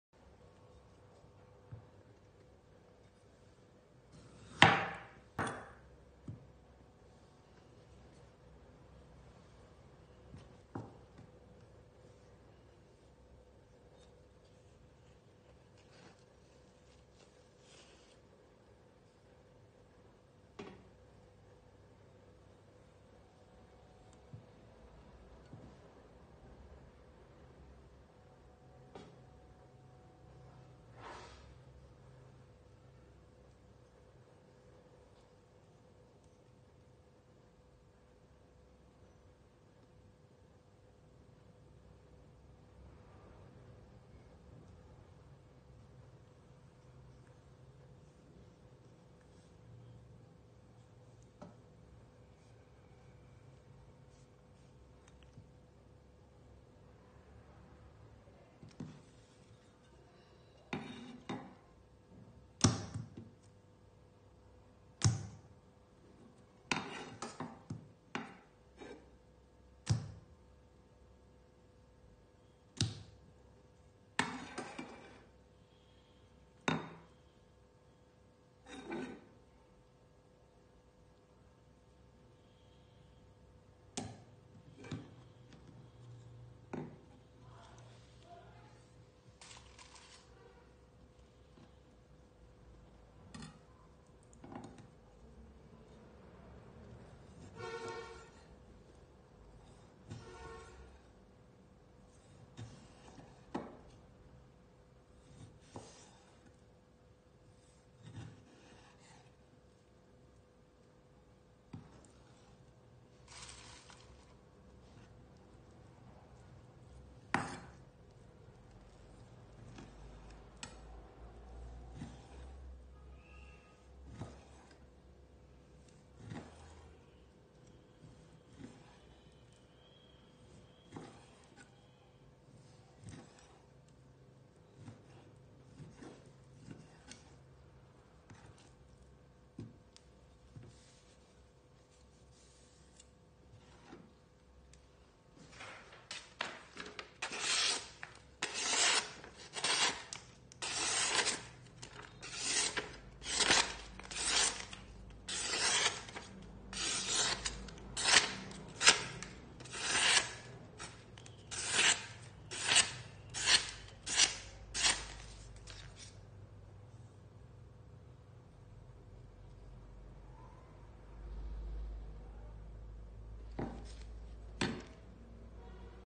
dao KAI Shun Kanso 8' - hàng lỗi đã bán hết
•Shun Kanso 8 "Con dao đầu bếp SWT0706
Lưỡi dao được chế tạo bằng thép không gỉ vanadi carbon cao tinh luyện AUS10A của Nhật Bản
• . Tay cầm được làm bằng gỗ tagayasan, đôi khi được gọi là "gỗ kiếm sắt". một loại gỗ cực kỳ dày đặc theo truyền thống được sử dụng để tạo ra tay cầm gỗ kiếm sắt Samurai
* dùng thái đa năng
800k
ib zalo- sdt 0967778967 kimloan 91